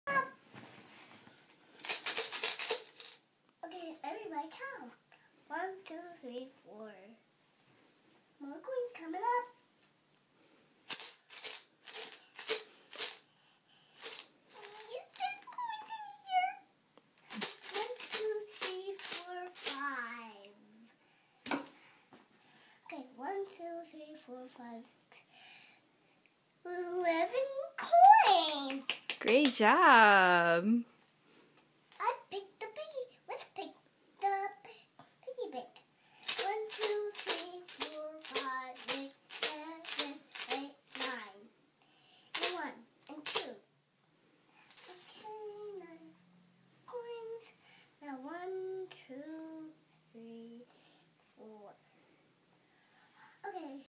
Zeia says "more coins comin up"
Some routine she has with her piggy bank and counting coins